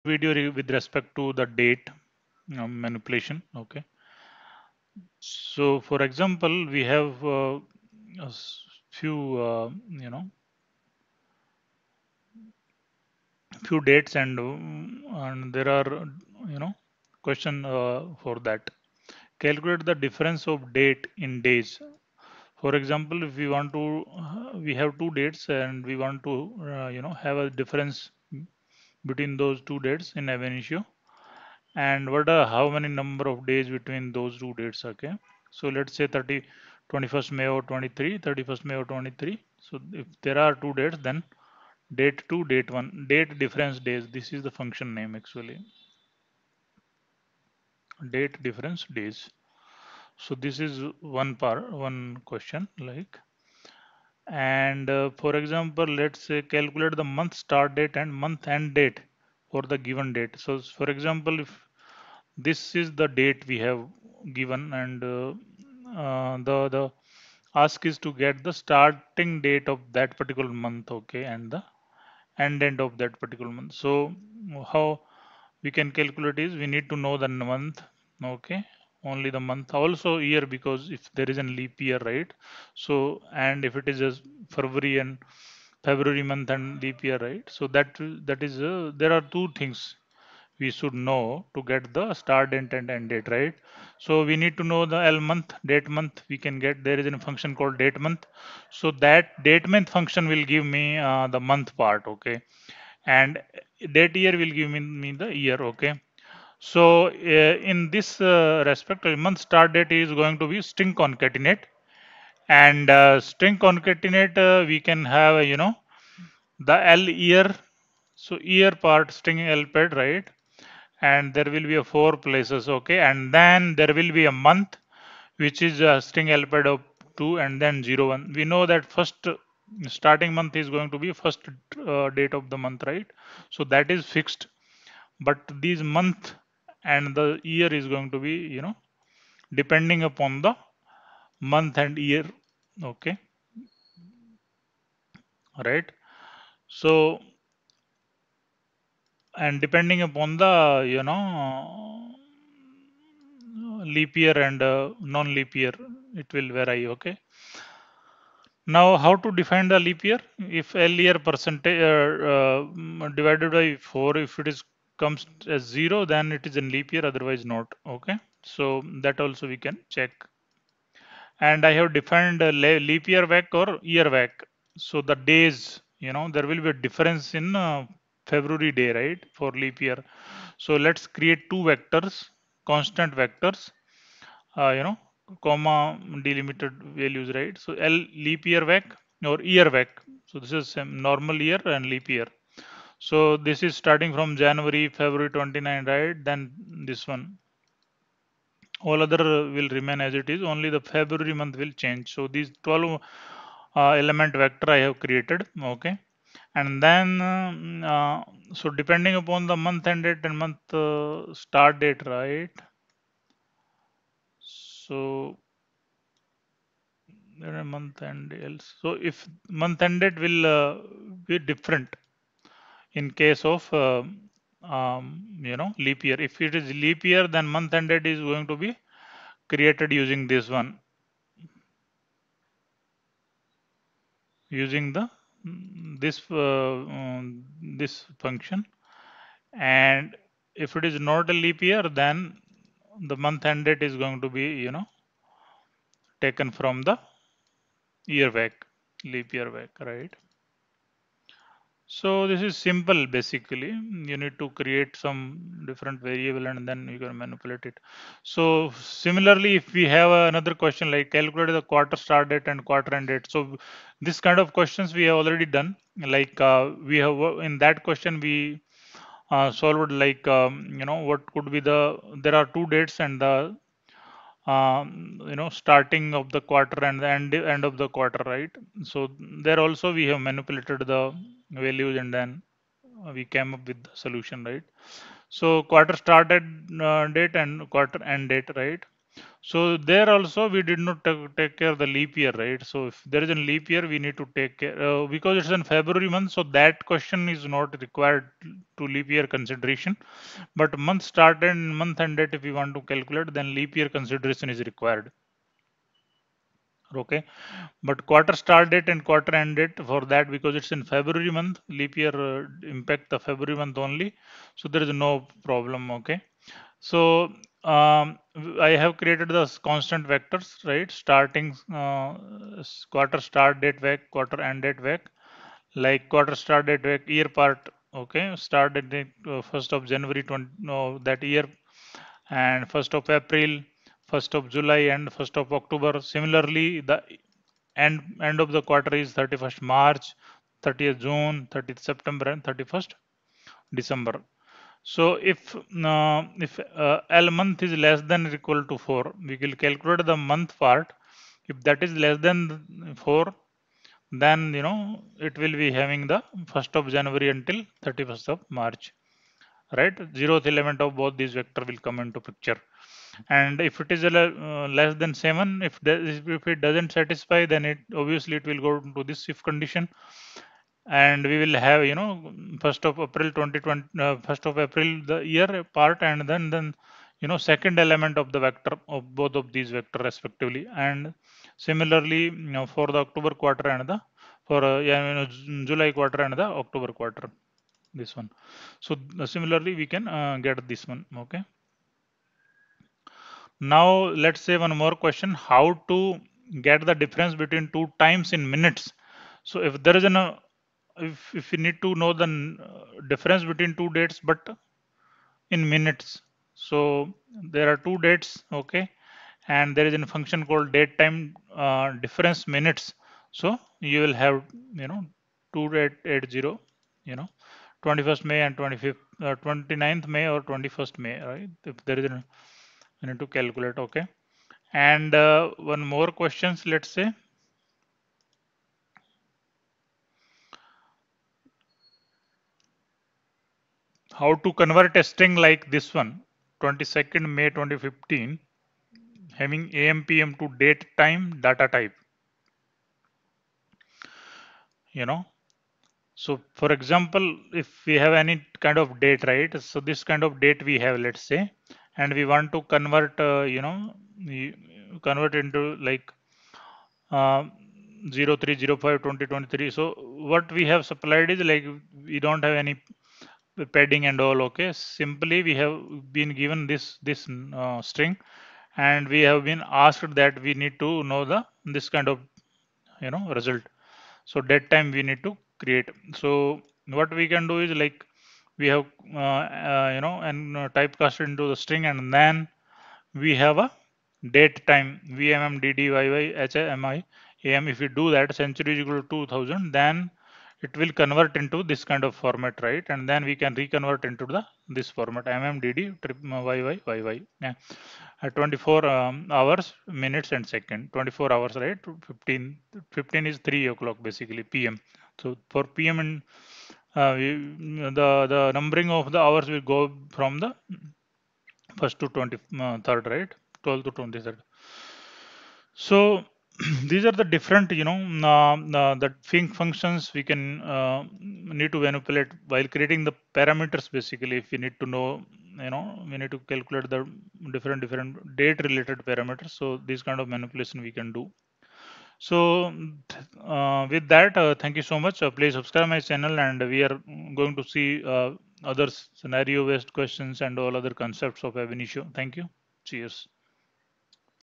Abinitio Interview Question 46
56.1  Calcucate the difference of date in days
56.2  Calculate the Month Start Date and Month End date for the given date
56.3  Calculate the Quarter start date and Quarter End Date for the given date
56.4  How to get the difference between 2 times in minutes
56.5 How to convert a string like "2/22/2015 9:54:02 AM" having AM/PM to DateTime datatype?